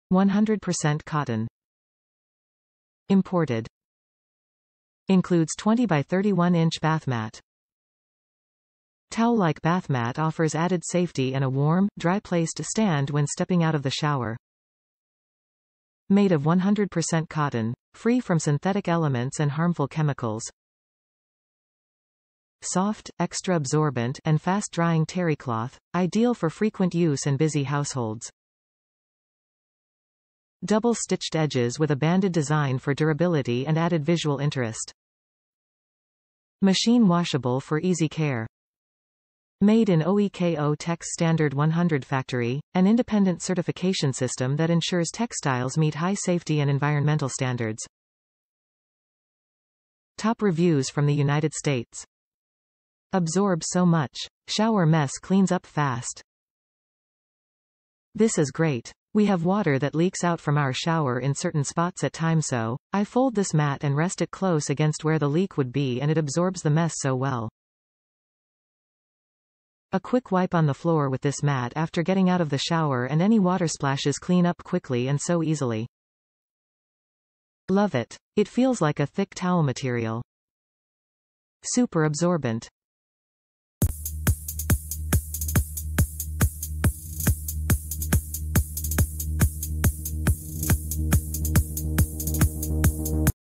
Amazon Basics Banded Bathroom Bath Rug Mat - 20 x 31 Inch, Powder Grey
100% Cotton
Imported
Includes 20 x 31 inch bath mat
Towel-like bath mat offers added safety and a warm, dry place to stand when stepping out of the shower
Made of 100% cotton; free from synthetic elements and harmful chemicals
Soft, extra absorbent, and fast-drying terrycloth—ideal for frequent use and busy households
Double-stitched edges with a banded design for durability and added visual interest
Machine washable for easy care
Made in OEKO-TEX Standard 100 factory, an independent certification system that ensures textiles meet high safety and environmental standards

Absorbs so much! Shower mess cleans up fast!
This is great! We have water that leaks out from our shower in certain spots at times so, I fold this mat and rest it close against where the leak would be and it absorbs the mess so well. A quick wipe on the floor with this mat after getting out of the shower and any water splashes clean up quickly and so easily. Love it! It feels like a thick towel material. Super absorbent!